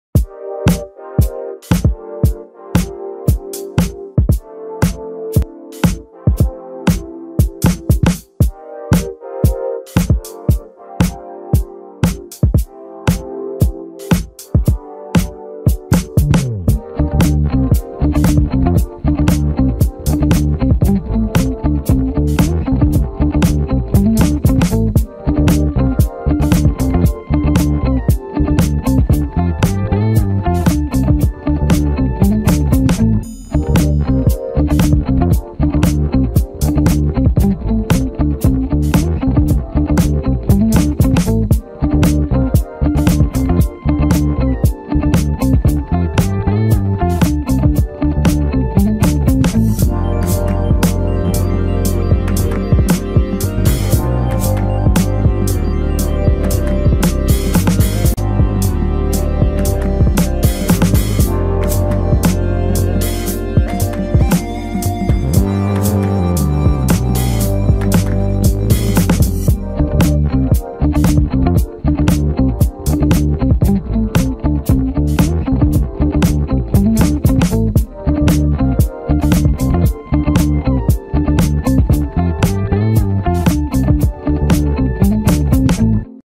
Paneer kheer - जन्मास्टमी में बनाइये स्वादिष्ट पनीर की खीर - Paneer Payasam recipe - cooklaplaza
My Setup
Photron Tripod

Kitchen Set
Pressure Cooker
Non-Stick Kitchen Set
Mixer Grinder

ingredients are:😋😍😋

cardamom 4-5 piece
sugar 50 gram
grated paneer 150 gram
almonds & cashewnuts chopped 2 tbp
raisins 9-10 piece